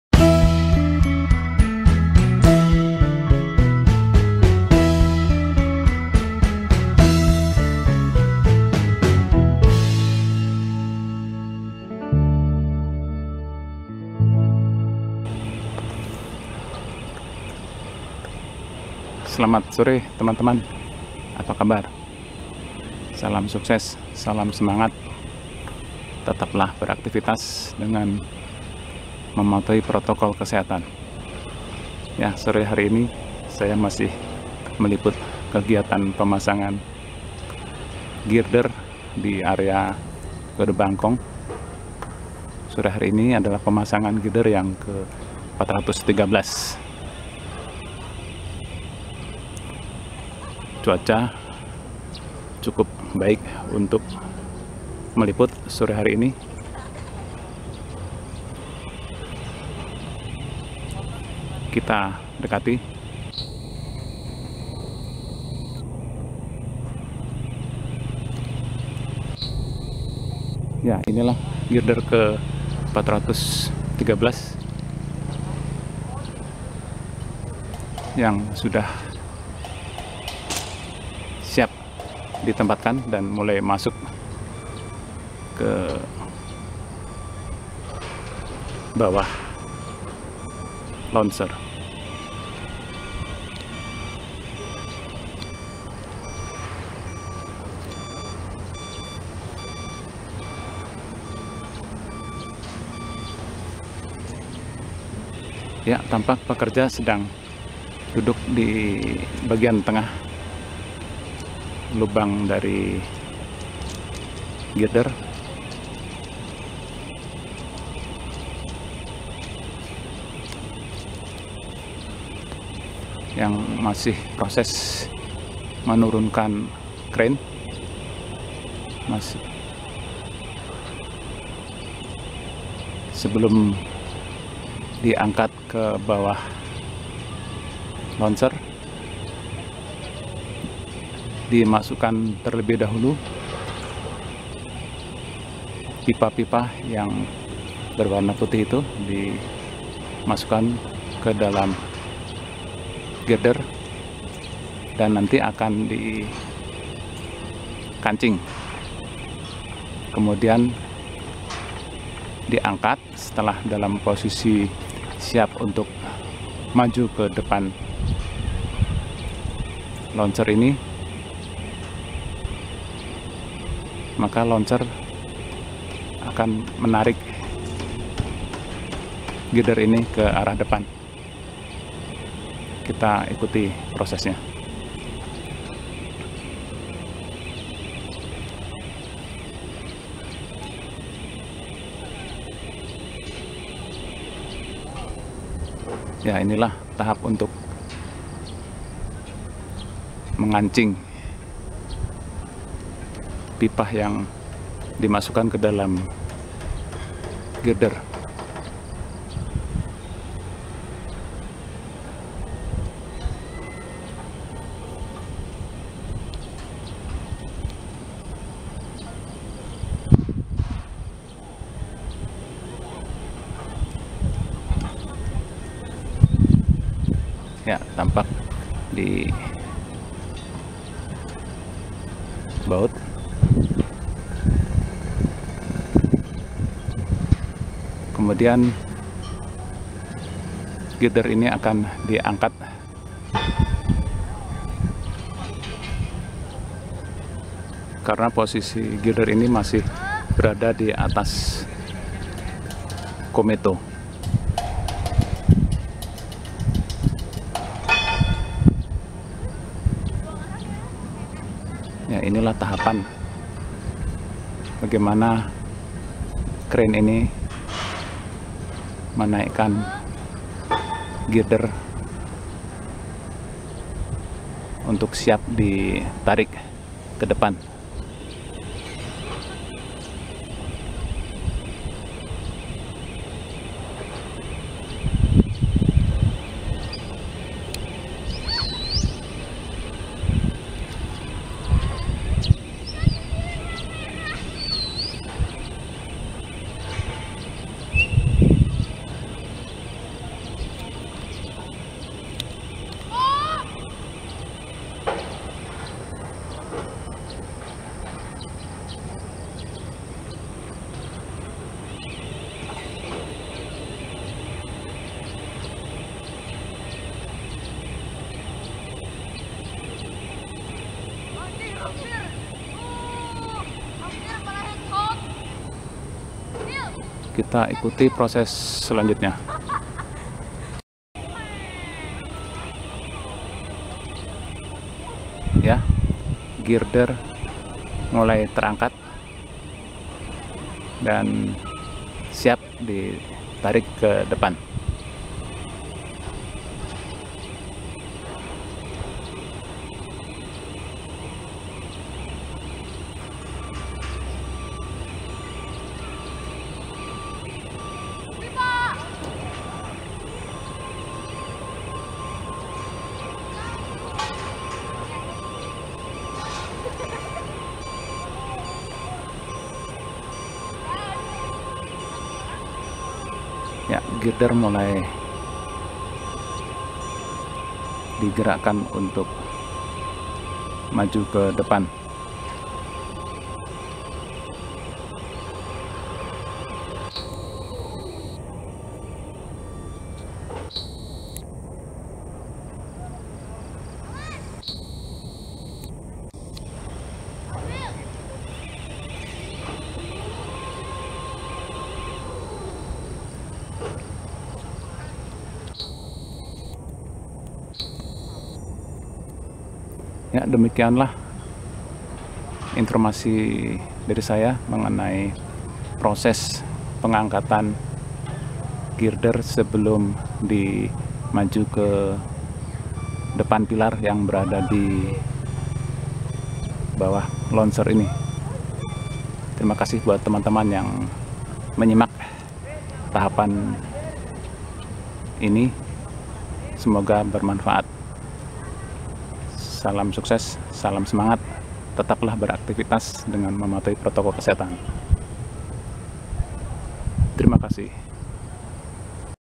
yuk intip!!! LUBANG GIRDER seHARI terPASANG 3 DI AREA BUKIT PERMATA BANDUNG BARAT up 23 Sep 2021
Inilah kegiatan pekerja pemasangan Girder ke 413 dan hari ini sudah terpasang 3 buah girder,
Tayangan kali ini melihat kegiatan para pekerja yang bertanggung jawab mempersiapkan pengancingan Girder pada crane sebelum dijalankan ke arah bawah mesin Launcher
Pemasangan terus dilakukan di area ini
Selamat menonton
Salam sukses sehat selalu